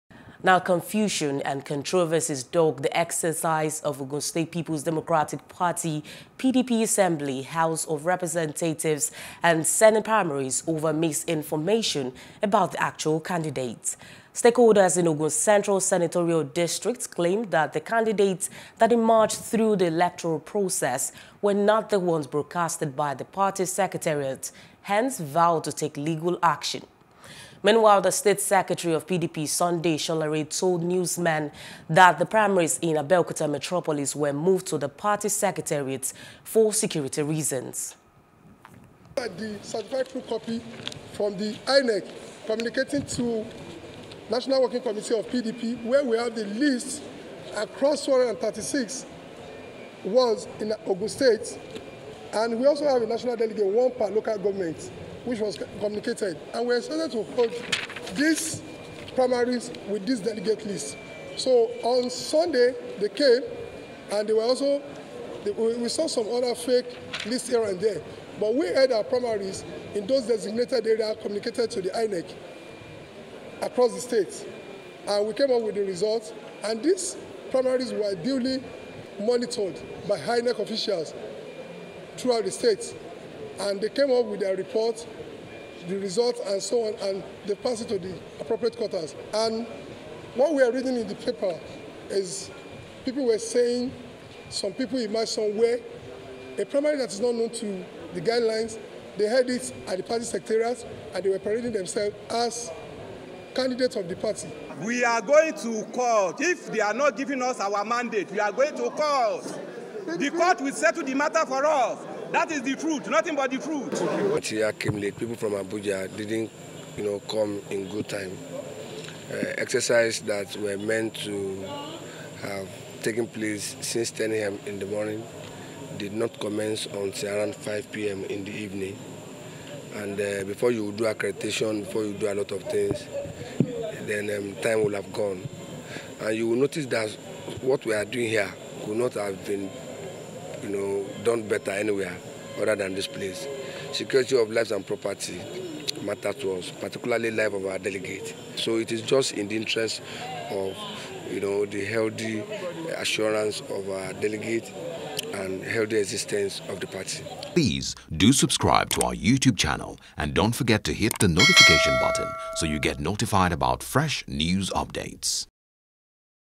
P.D.P. Primaries: Stakeholders Threaten Court Action Over Misinformation Of Candidate | NEWS
Confusion and Controversies dogged the exercise of Ogun State People's Democratic Party (PDP) Assembly, House of Representatives, and Senate Primaries over misinformation about the actual candidate.